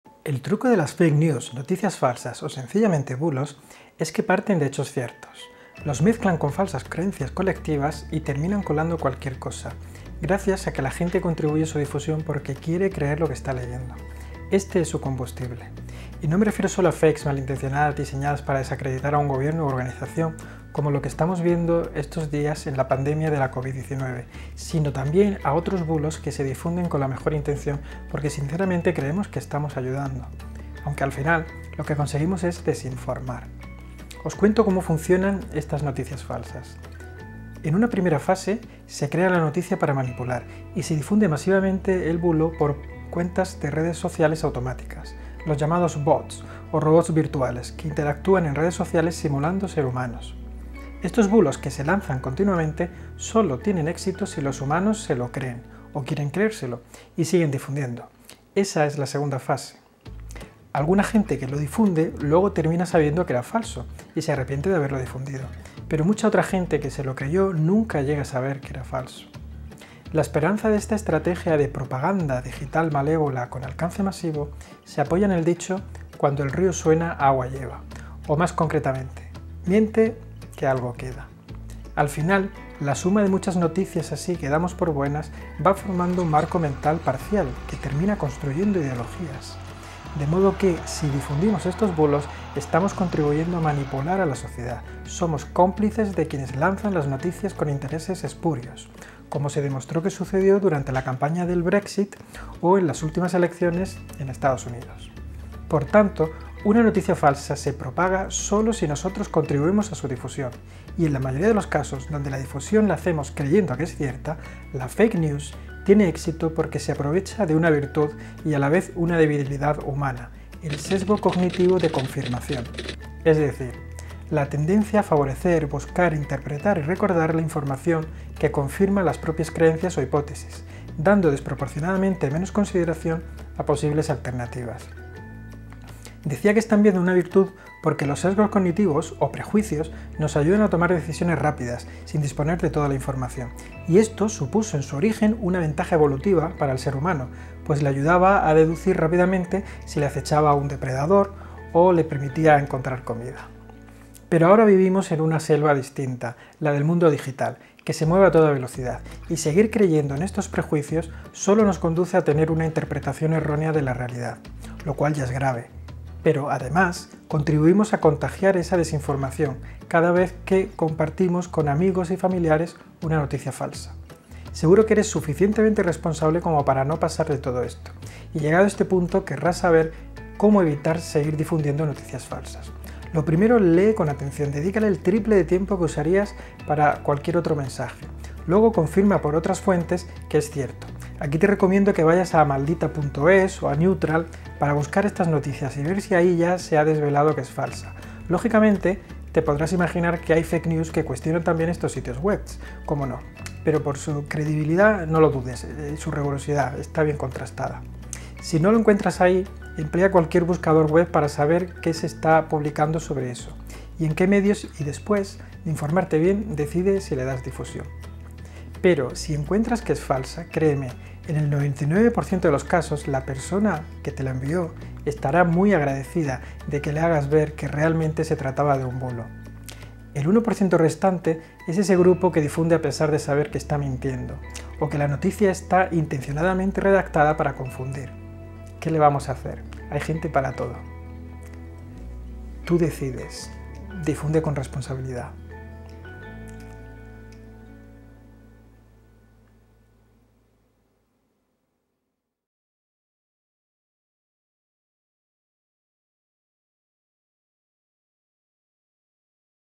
Fake news: qué son, cómo funcionan y cómo evitarlas
El truco de las fake news, noticias falsas o, sencillamente, bulos, es que parten de hechos ciertos, los mezclan con falsas creencias colectivas y terminan colando cualquier cosa gracias a que la gente contribuye a su difusión porque quiere creer lo que está leyendo. Este es su combustible.

Estos bulos que se lanzan continuamente solo tienen éxito si los humanos se lo creen (o quieren creérselo) y siguen difundiendo, esa es la segunda fase. Para conseguir esto, la fake news se aprovecha de una debilidad humana: el sesgo cognitivo de confirmación.

Para evitar seguir difundiendo bulos, confirma por otras fuentes su veracidad. Aquí te recomiendo que vayas a o para buscar estas noticias y ver si ahí ya se ha desvelado que es falsa.

Si encuentras que es falsa, en el 99% de los casos, la persona que te la envió te estará muy agradecida de que le hagas ver que realmente se trataba de un bulo. El 1% restante es ese grupo que difunde a pesar de saber que está mintiendo, o que la noticia está intencionadamente redactada para confundir. ¡Qué le vamos a hacer, hay gente para todo!

Tú decides, difunde con responsabilidad.